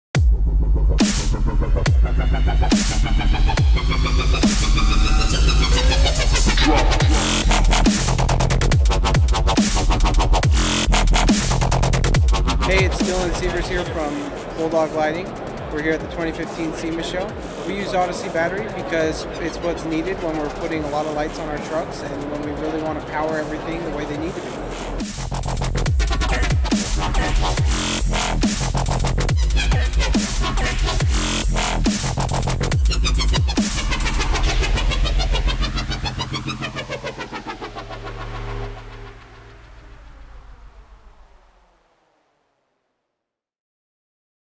SEMA 2015 Build: Dylan Sievers
Listen to why Dylan Sievers of Bulldog LED Lighting uses ODYSSEY in all of his trucks and builds!